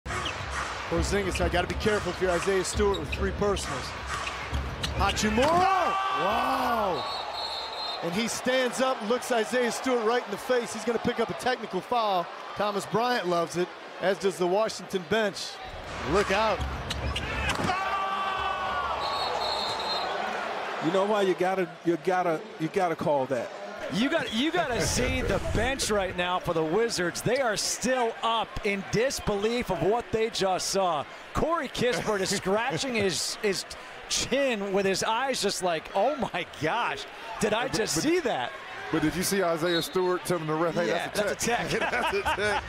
Isaiah Stewart REQUESTS for a tech on Hachimura after Wizards bench embarrasses him on a POSTER!
Appreciate it! 😊